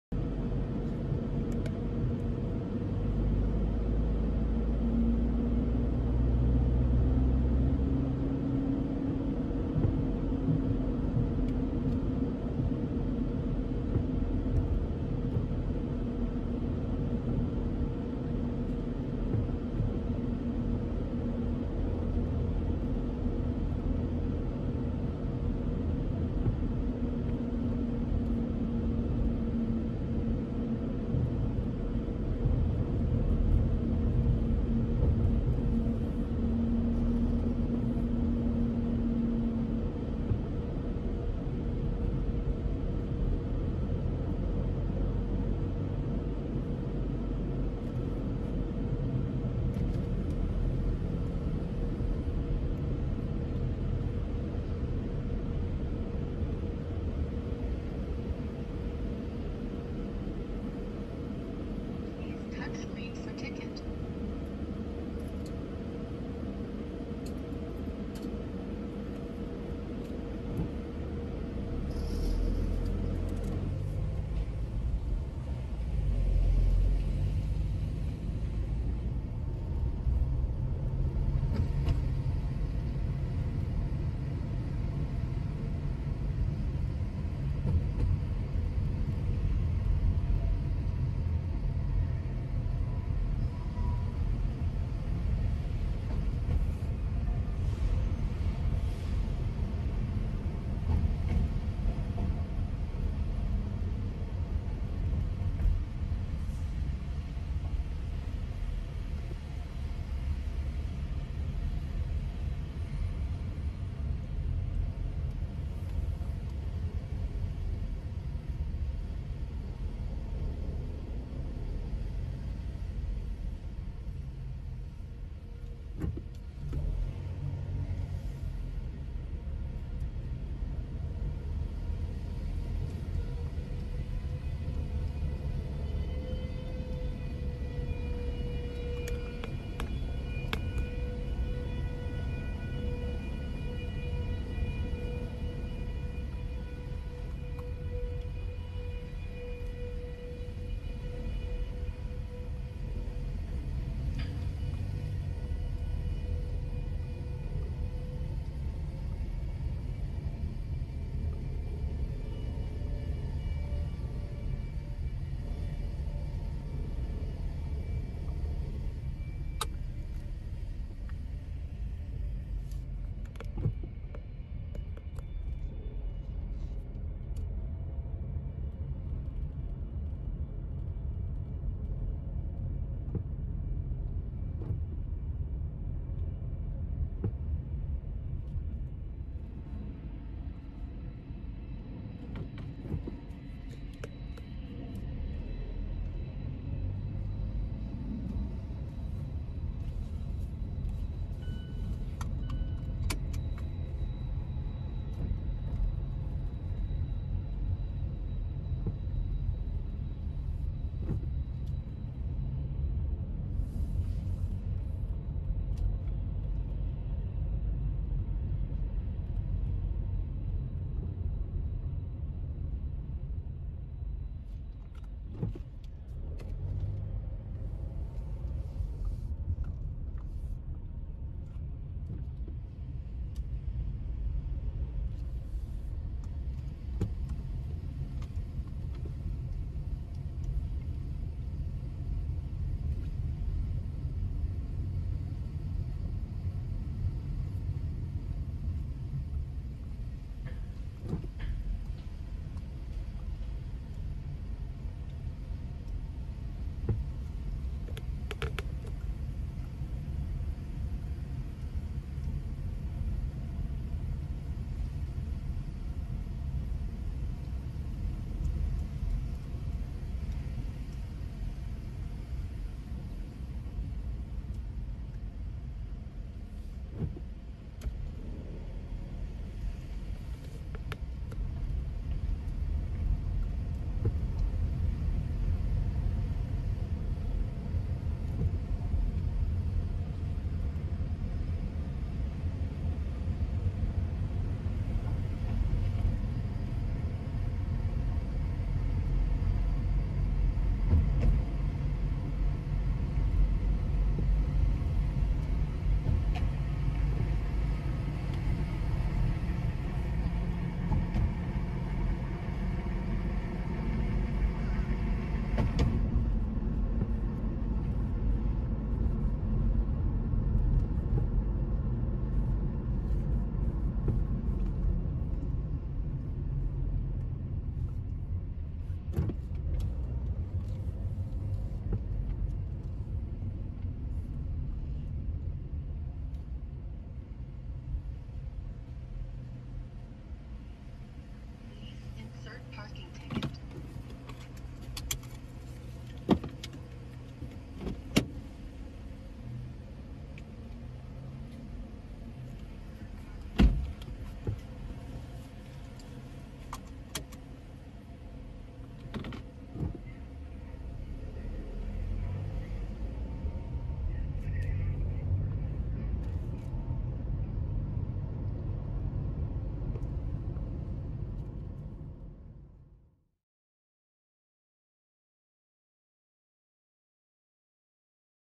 Tampa International Airport - Short Term Parking Drive - Entrance and Exit
Parking is VERY easy here. One of the easiest airports in the country to navigate. Tampa International Airport is a Gem in a world of confusing and crowded airports. Getting in and out is quite simple.